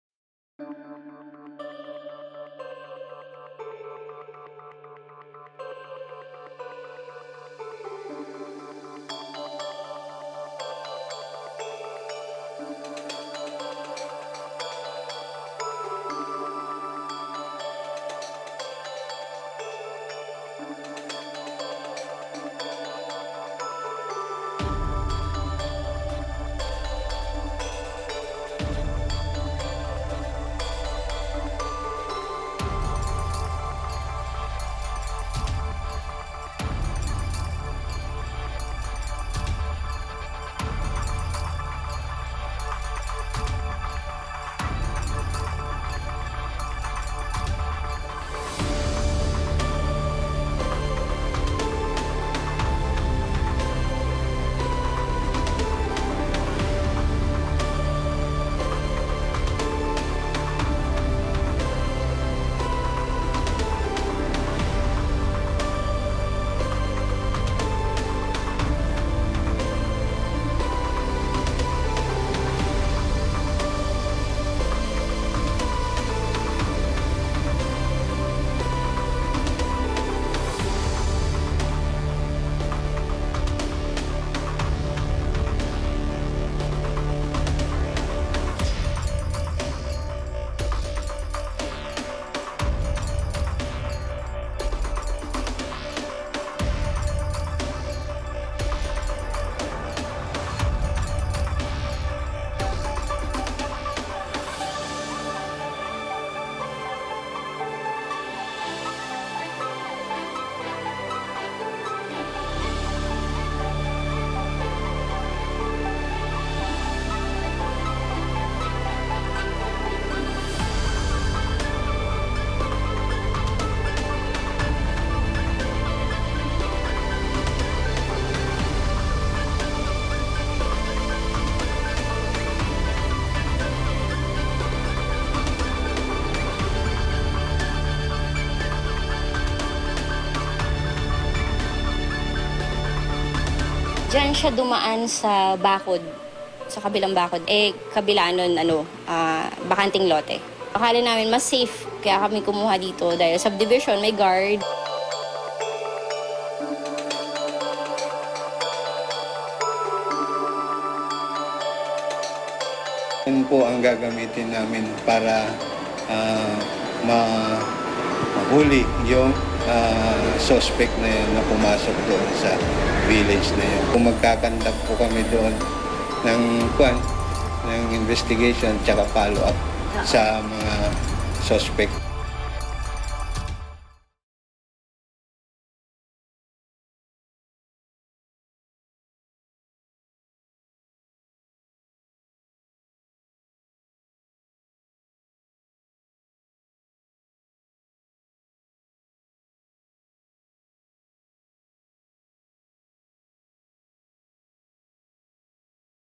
Nakawan sa Taguig
Huli sa CCTV ang magkabilang nakawan sa isang subdivision at isang kainan sa Taguig.